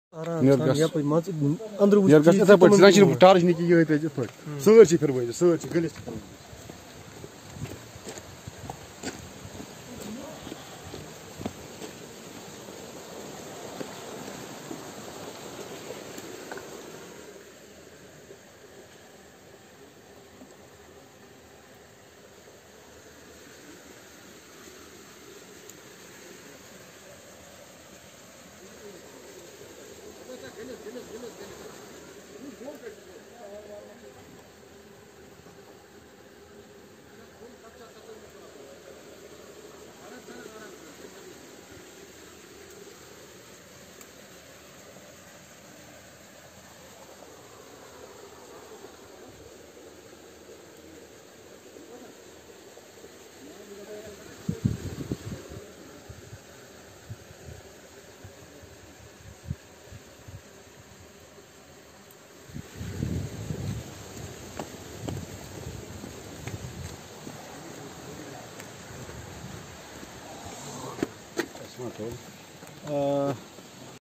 Forces Recovered an (IED) from a Car (Santro ) which has been Blasted by Bomb disposal squad
Forces Recovered an Impressive Explosive Device (IED) from a Car (Santro ) which has been Blasted by Bomb disposal squad of Army and Police  in Avindgund Rajpora Pulwama.